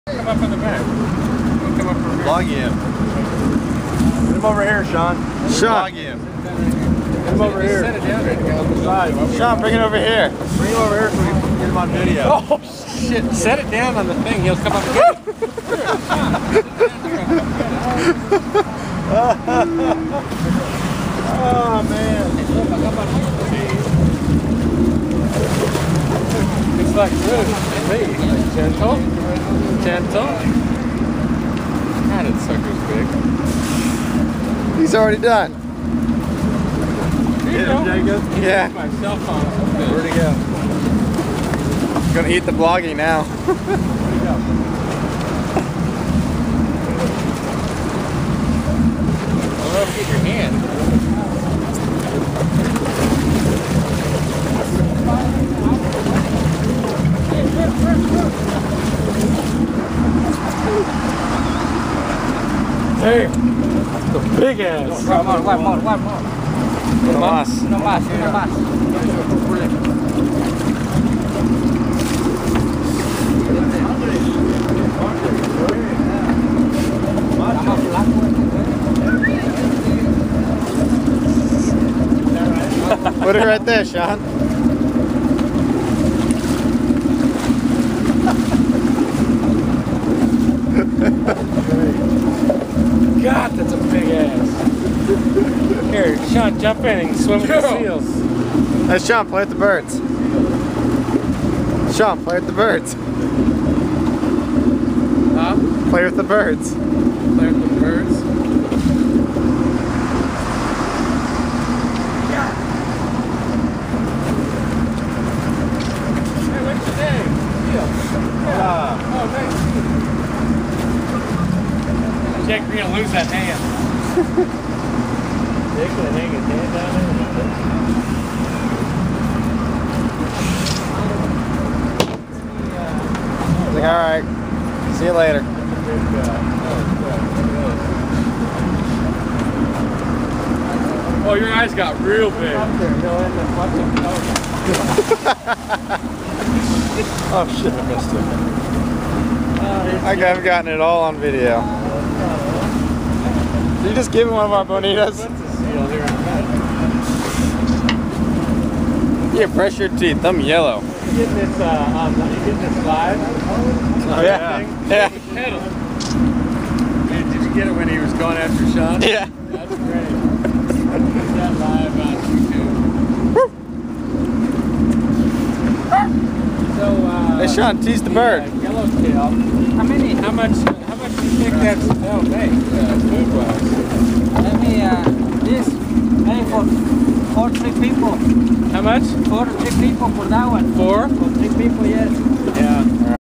Almost losing a hand to a sea lion
Feeding fish to a sea lion while my Camera almost get eaten by a 400lb sea lion and my friend almost loses his hand